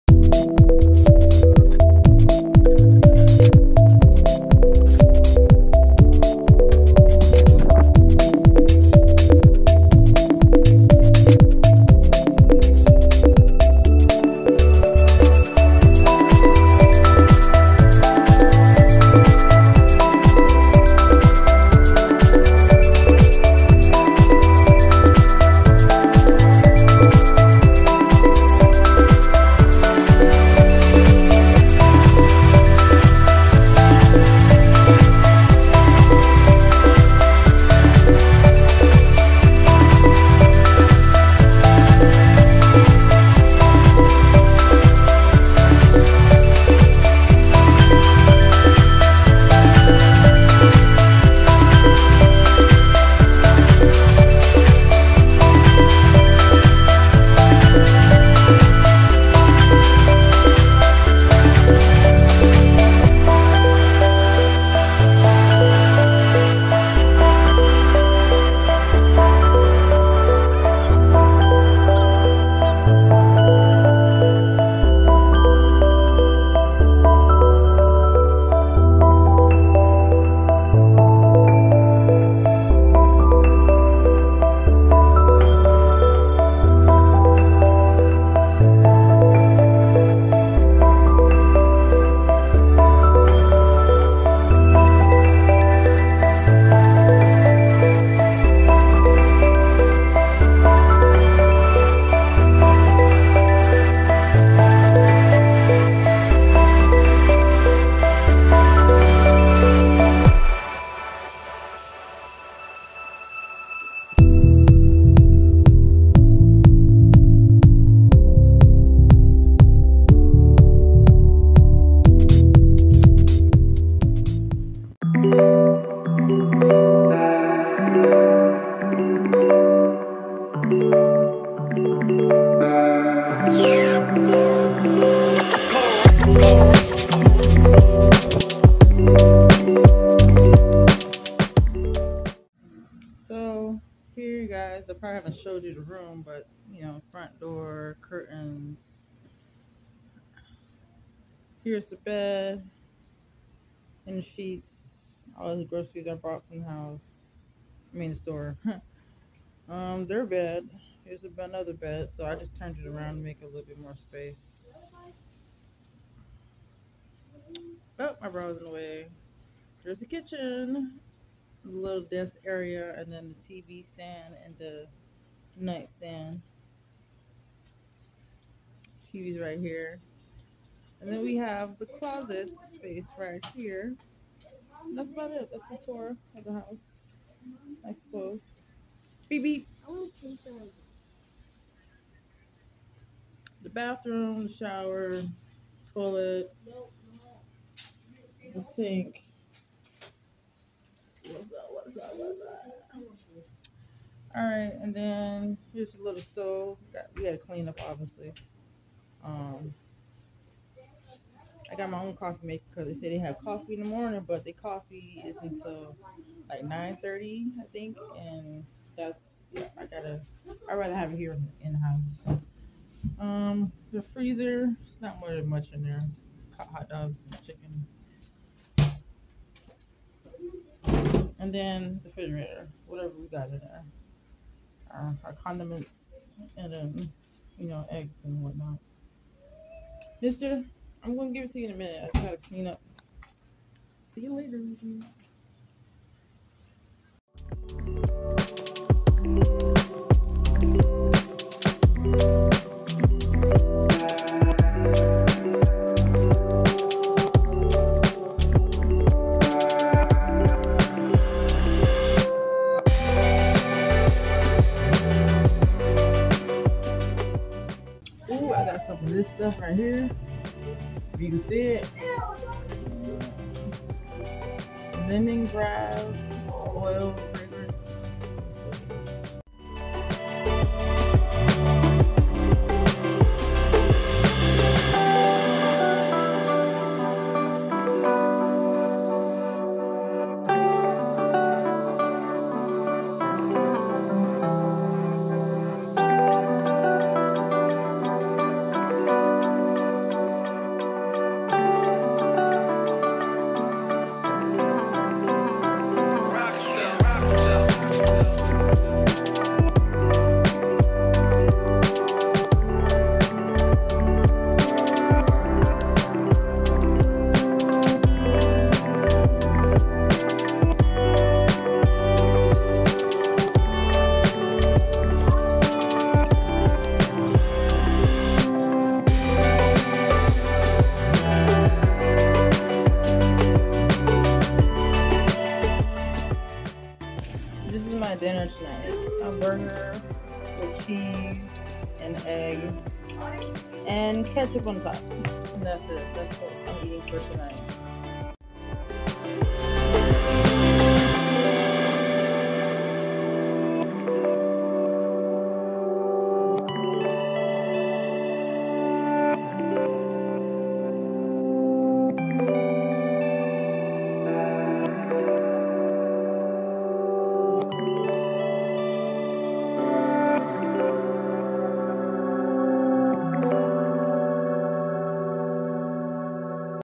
Our last at the house and staying at a hotel 🛏️🛍️
Good * morning
          * afternoon
          * evening

Hope all is well with everyone. where are you at the living and still kind of having delays with trying to find somewhere to stay... I'm in the hotel for 2 weeks but I'm not sure how that's going to work out for me with my case being open because if I don't have room over my head my kids get taken away from me again and I really don't want that to happen.

I have court coming up this Tuesday so let's see what happens... GoD is with Us...

* The Fragrance oil (Aromor) Lemongrass, can be found at Family Dollar or Bravos grocery store... I'm not sure if all grocery stores sell them but I know that the stores I went to have them so check them out good fragrance in the house to smell good.